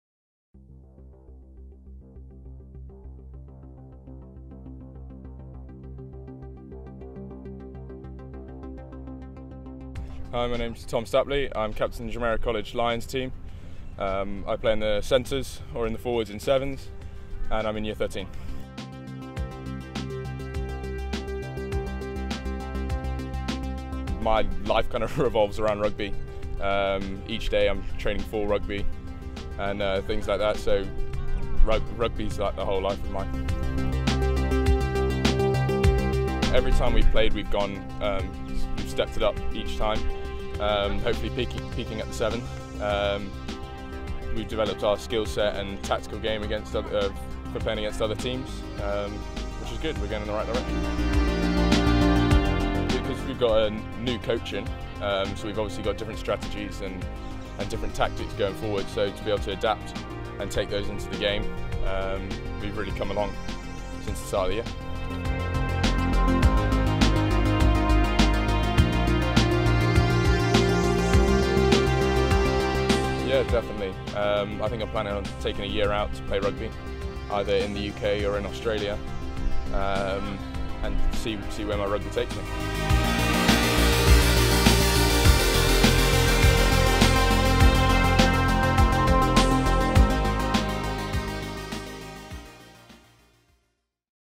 Jumeirah College Takes on the Dubai Rugby Sevens | Sun & Sand Sports Blog
Sun & Sand Sports follows the Jumeirah College Rugby team on their quest for glory at this year’s Dubai Rugby Sevens tournament.